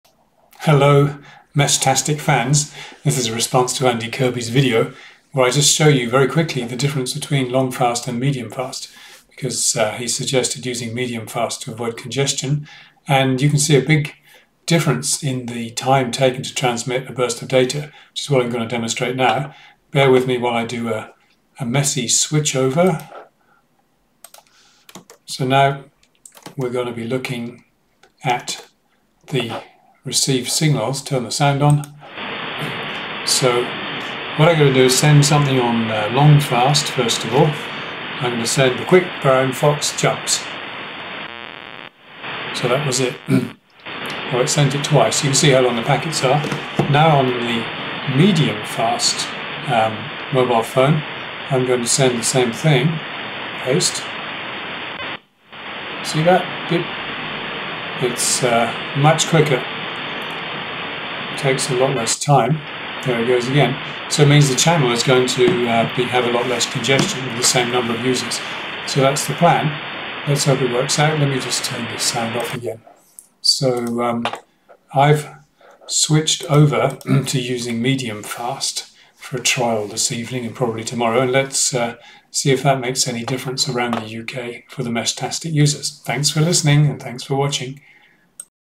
MediumFast Meshtastic
Andy Kirby in the UK has suggested using MediumFast Modem Preset in the Lora settings in order to reduce congestion in densely used areas. There is a link budget penalty to pay for this but it may be outweighed by the advantage gained by using shorter bursts. My video shows these bursts using an SDR receiver.